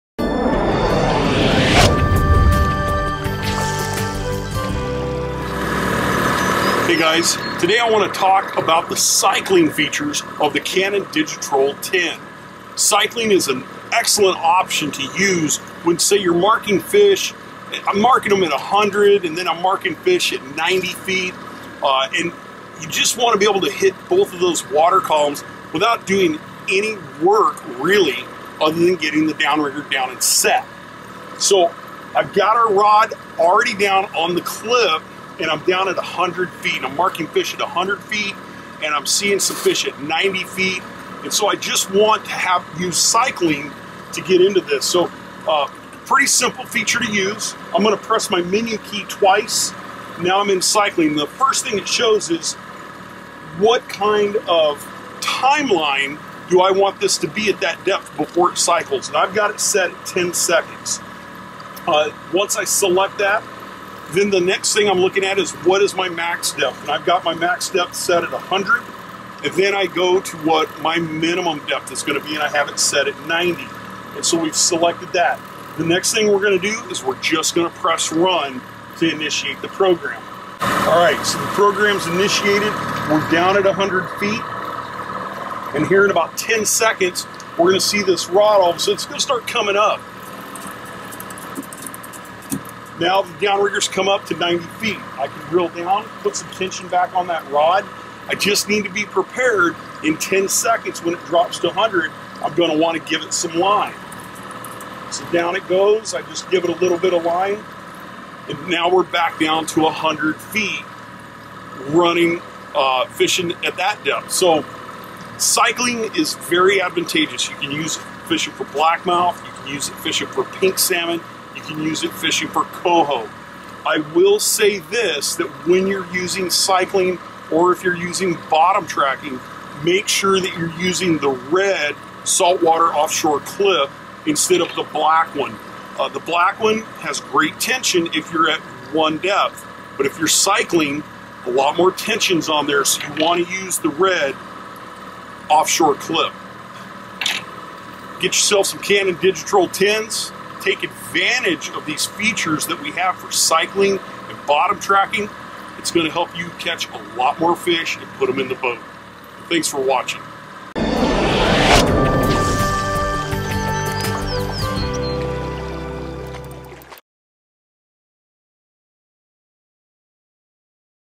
How to Cycle Depths on Downriggers | Cannon Depth Cycling
The Depth Cycling feature on the Cannon Digi-Troll 10 electric downrigger allows you to automatically cycle your tackle up and down between two desired depth levels at preset time intervals.

This is a great feature for tough days on the water where you can seem to get fish to bite because it gives a natural moving action to your bait and allows you to cover two different parts of the water column to put your bait in front of the fish.

Cannon Pro, Doug Saint-Denis walks you through why you should be using the Depth Cycling feature and how to set it up.